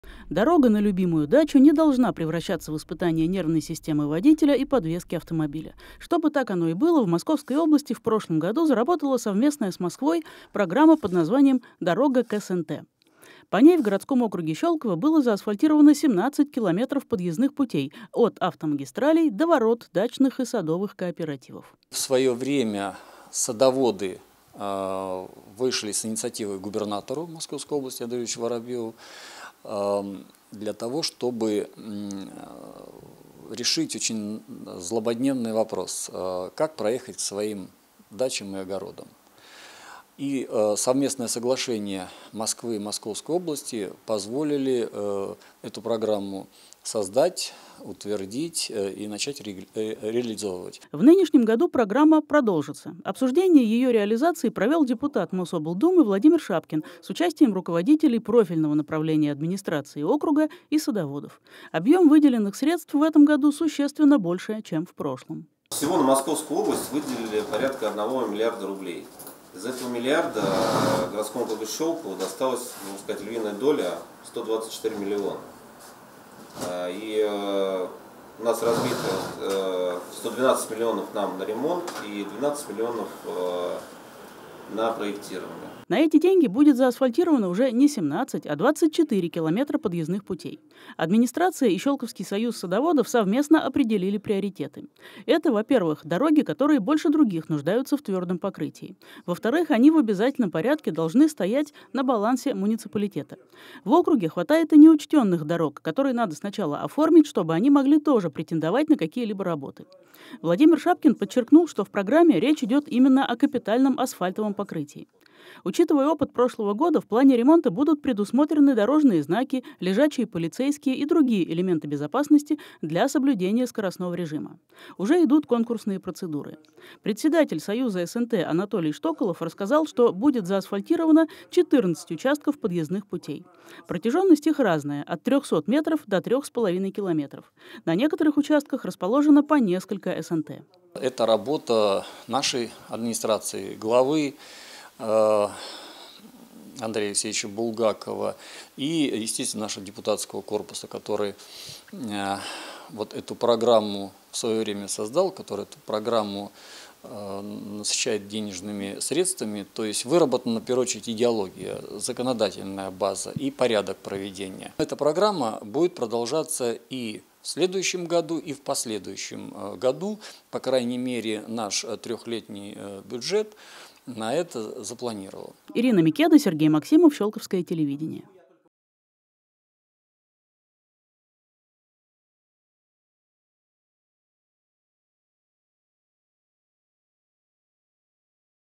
14 дорог к щелковским СНТ заасфальтируют до конца дачного сезона
24 километра подъездных путей к садовым и дачным кооперативам будут отремонтированы в этом сезоне по программе «Дорога к СНТ». Обсуждение реализации программы провёл депутат Мособлдумы Владимир Шапкин.